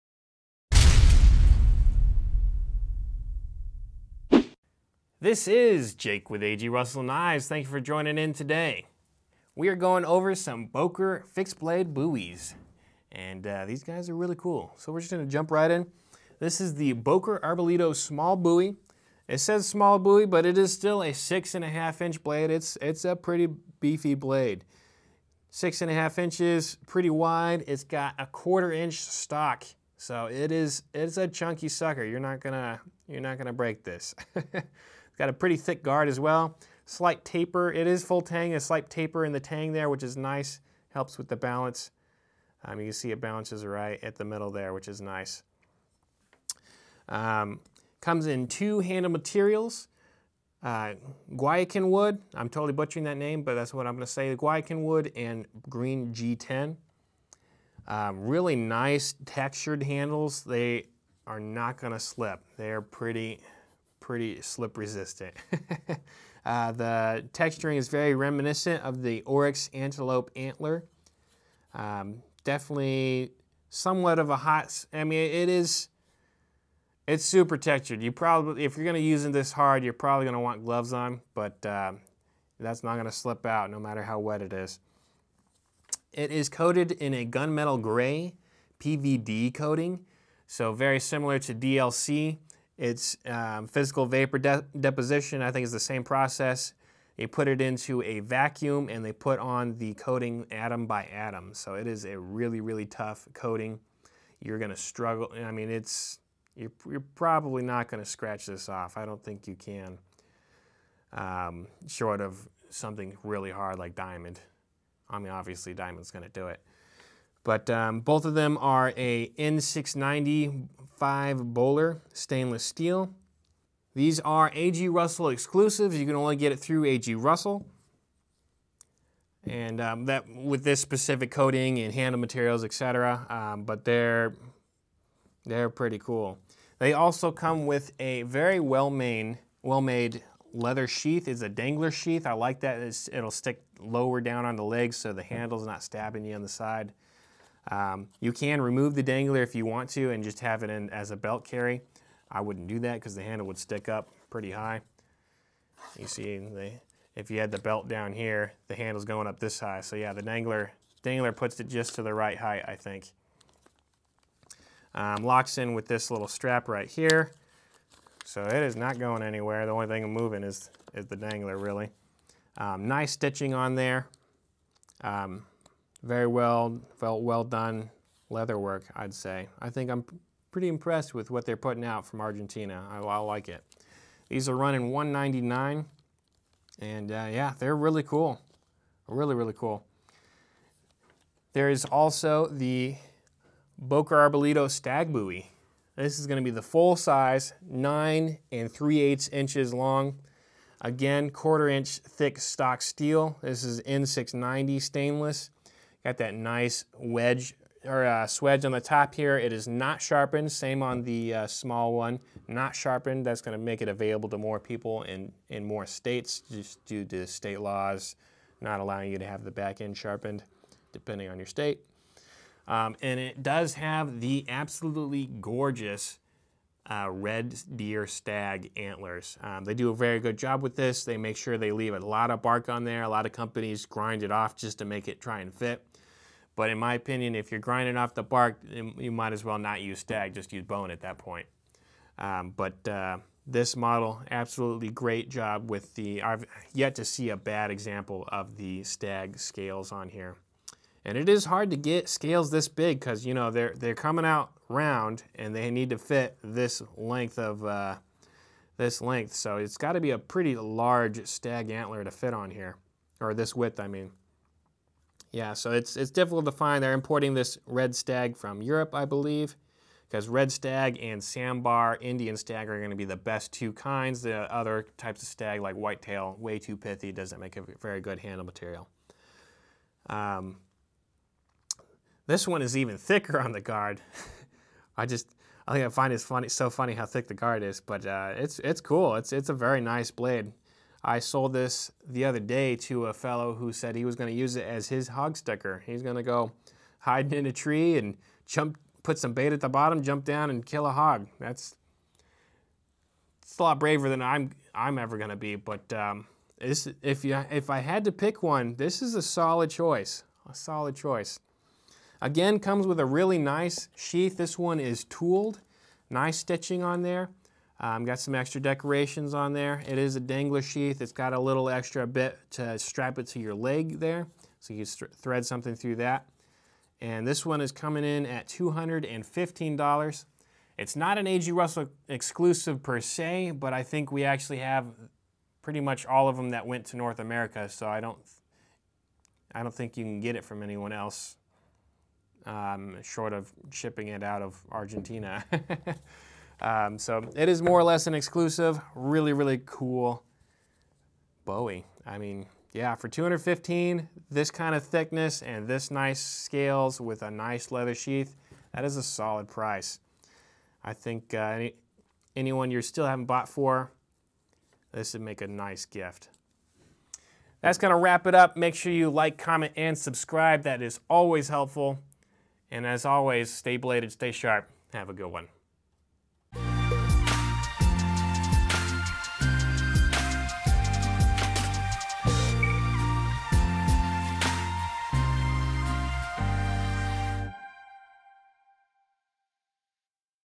Boker Arbolito Bowie Knives - El Gigante and Esculta Overview
Jake overviews three Boker Arbolito Bowies. Two Boker Esculta "small" 6-1/2" bowies with Guayacan wood or Green G-10 handles, and the Boker El Gigante, a large 9-3/8" stag handle bowie. All three have well made brown leather sheaths. This are big, meaty 1/4" thick stock fixed blades. The Small bowies have a gunmetal blue PVD coating over their Bohler N695 stainless steel and are exclusive to A.G. Russell Knives. The large bowie has Bohler N695 stainless steel, stag handle scales, a tooled leather sheath, and A.G. Russell has the vast majority of stock that went to North America.